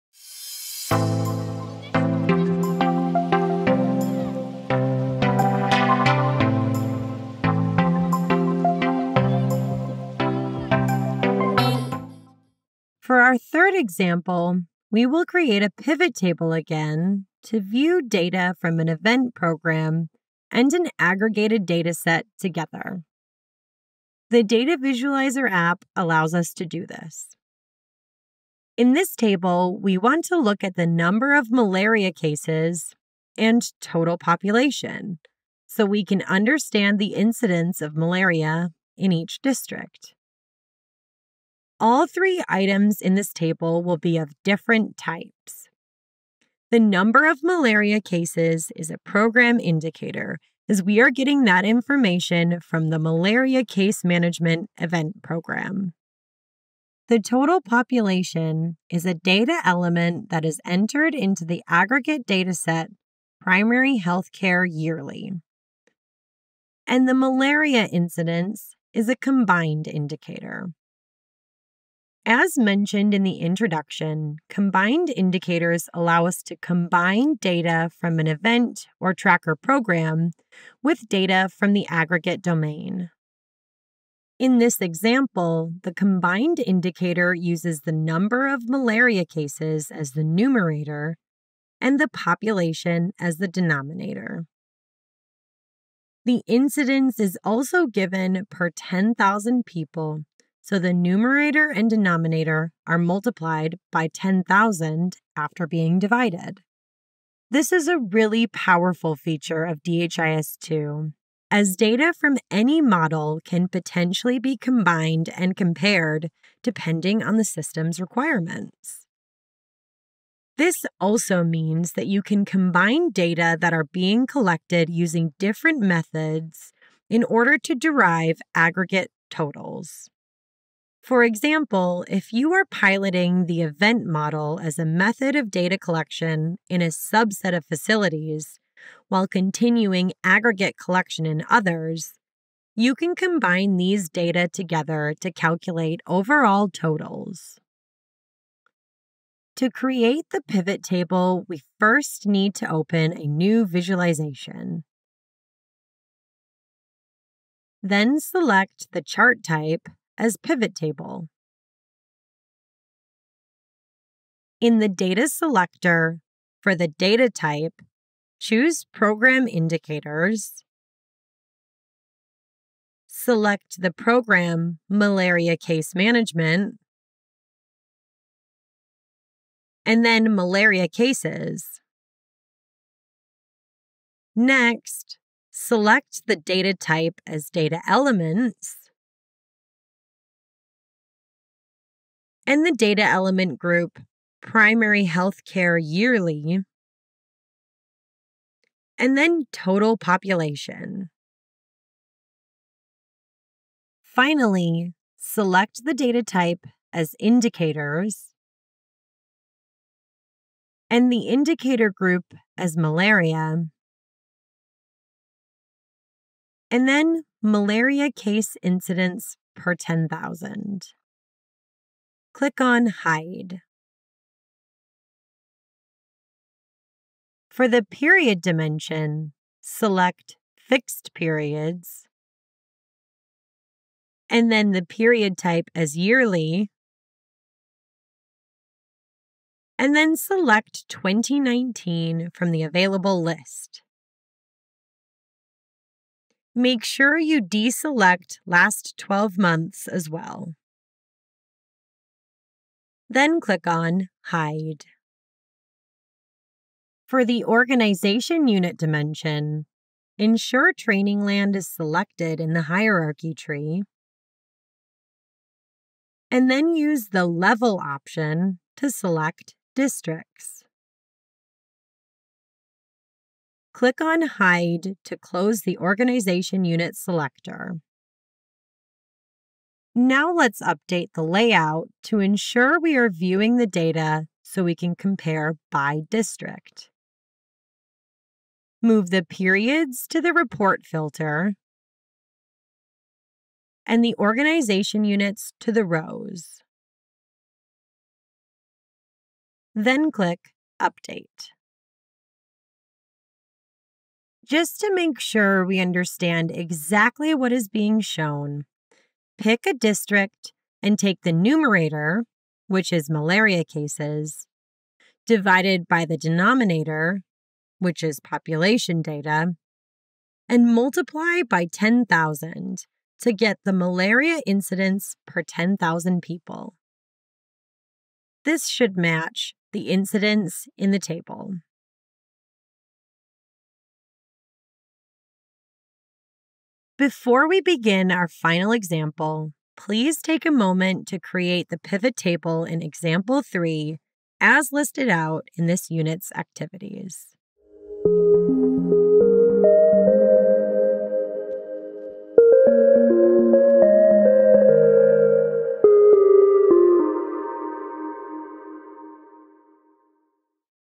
Module 4: Subsection 4 - Joint Analysis of Event & Aggregate Data Demonstration [Part 4 of 5]
[Part 4 of 5] This demonstration gives an overview of how event and aggregate data can be analyzed jointly using the Data Visualizer and Maps apps through indicators and program indicators. A demonstration is given on how this is done.